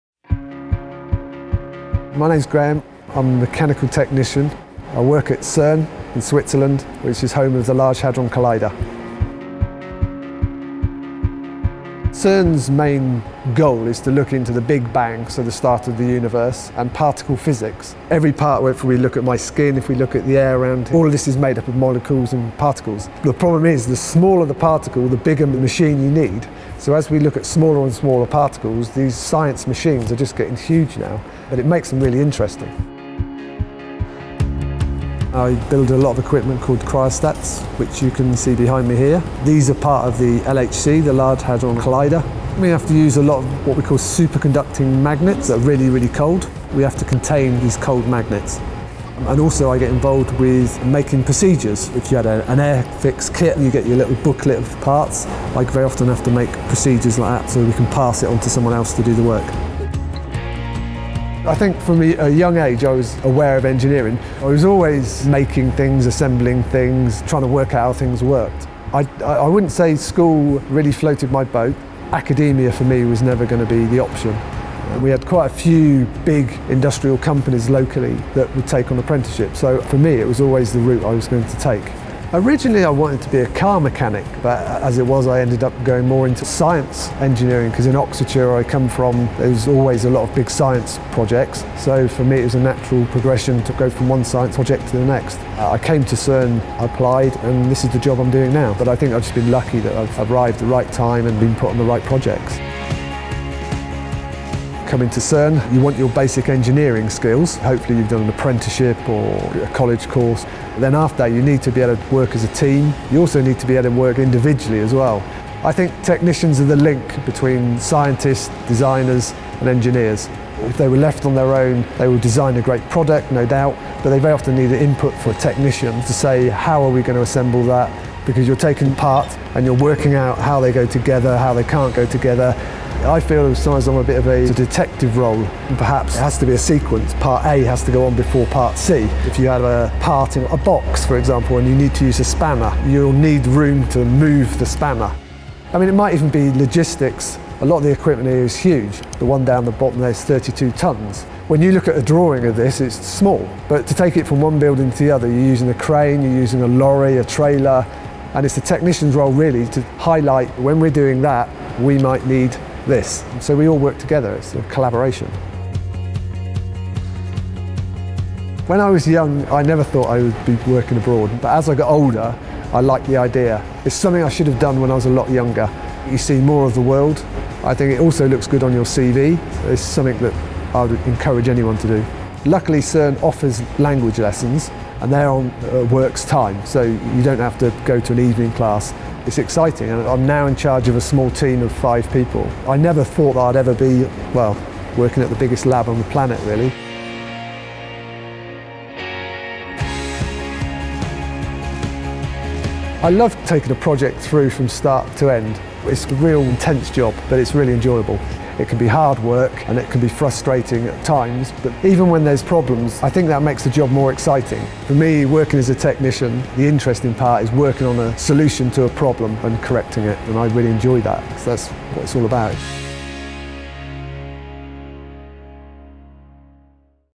Technicians Make It Happen - Meet Graeme
I think that from a young age, I was aware of engineering. I was always making things, assembling things, trying to work out how things worked."

Graeme, a Mechanical Technician working at CERN, explains that the most important thing about being a technician is working in a team collaboratively.

Video © Technicians Make It Happen (TMIH)